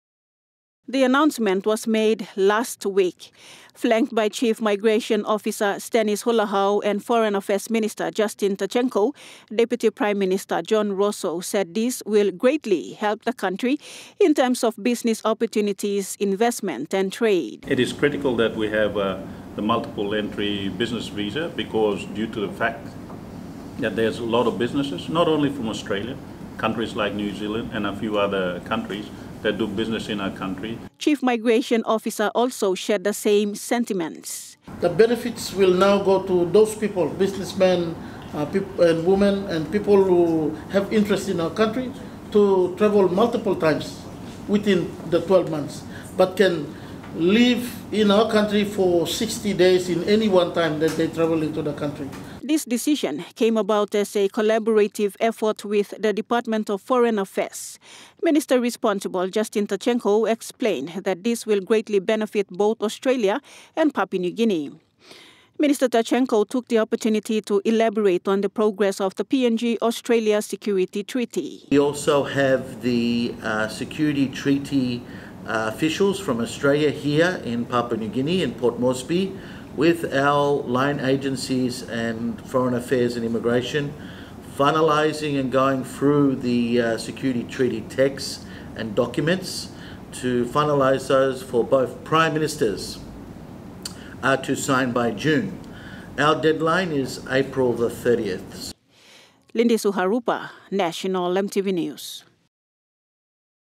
BUSINESS VISA
The Short Term Business Visa has been activated by the department of Immigration Citizenship Authority this month.
According to Deputy Prime Minister and Minister responsible for Immigration and Citizenship Authority, John Rosso, this will bring in more trade and investment into the country.